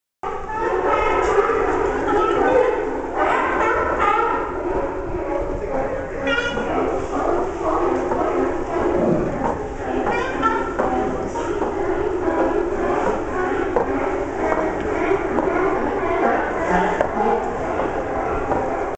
Sea Lions under Santa Cruz Wharf, Some swiming in water, ALSO......
Sea Lions under Santa Cruz Wharf, Some swimming in water, ALSO. The one seal you can get real close to. at end of video You'll see,